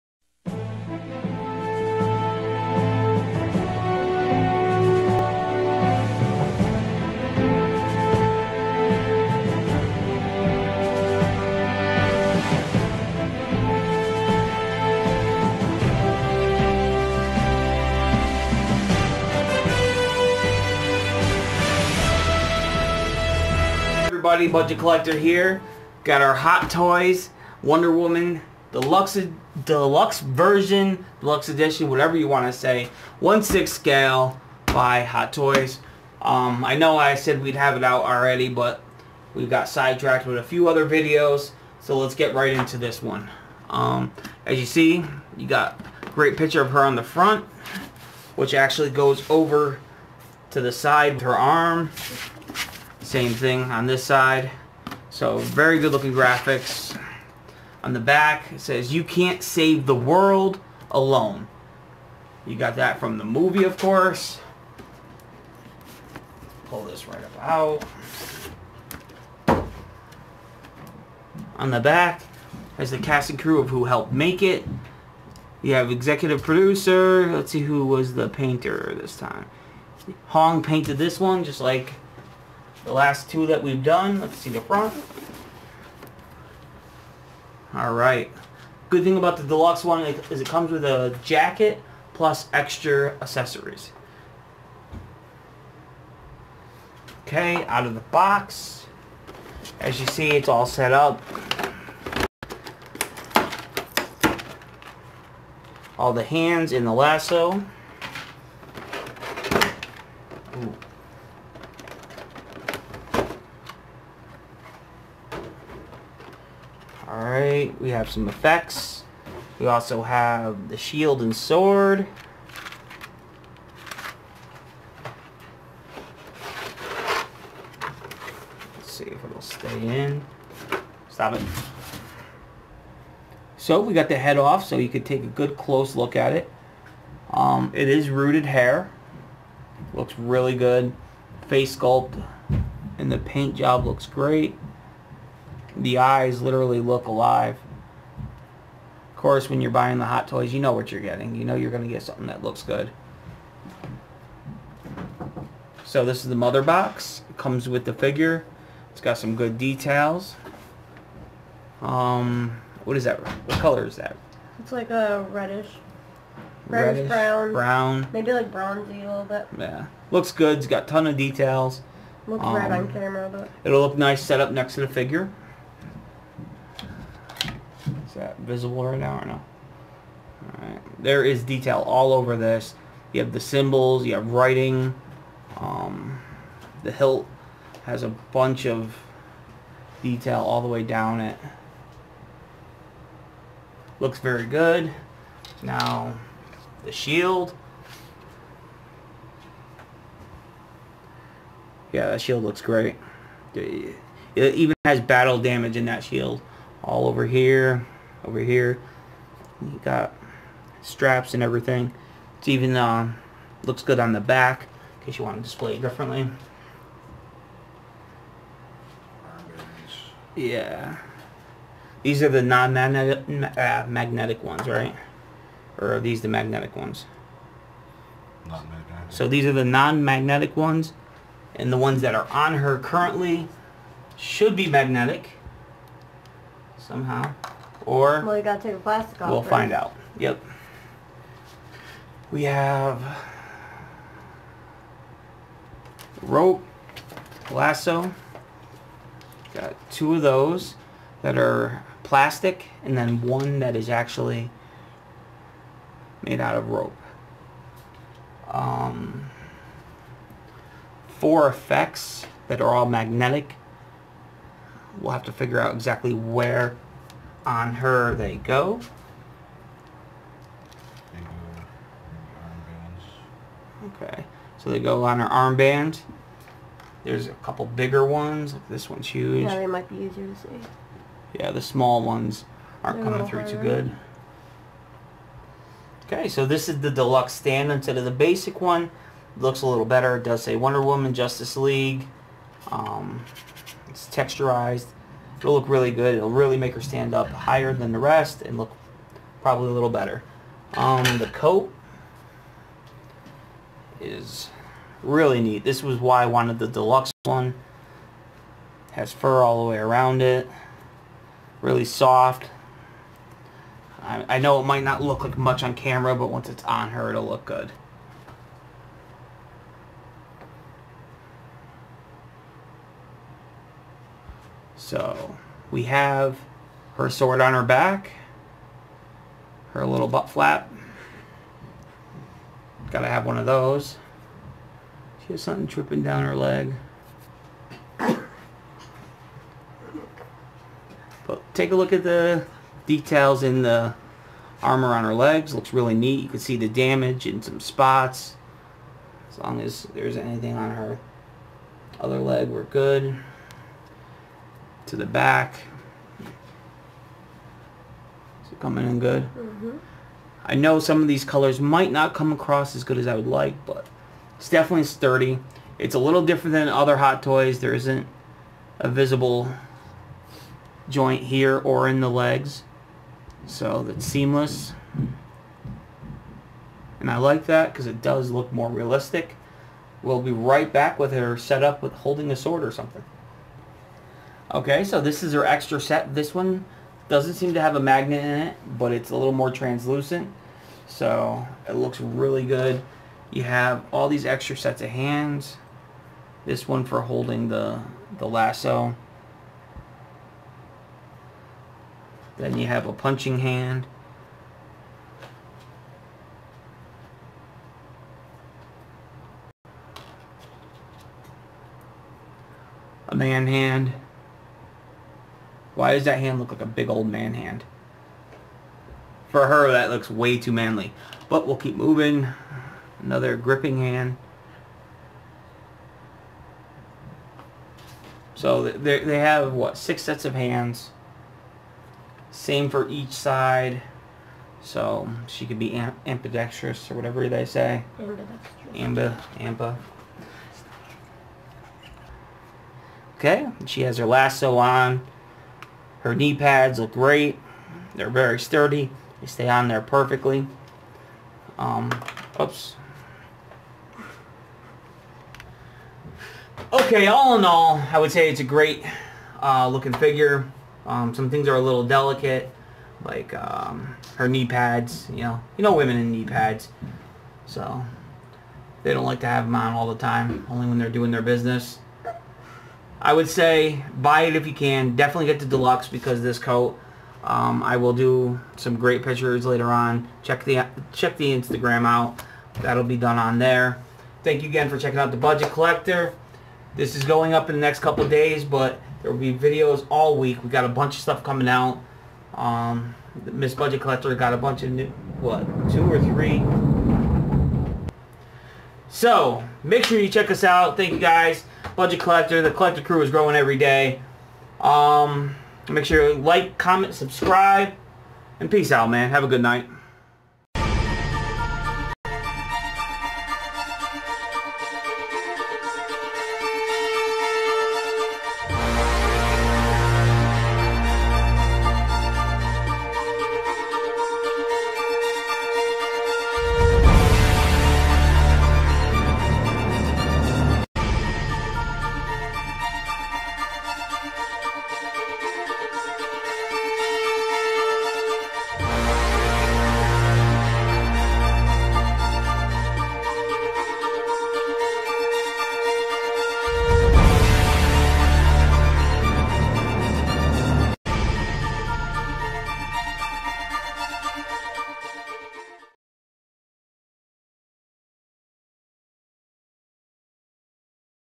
Justice League Wonder Woman Tossing Toys Part 3
An unboxing of the new Justice league Wonder woman hot toy deluxe. Just having a little fun with the new figure and learning how to break it as I go. Joining The Collector crew is making us stronger every day.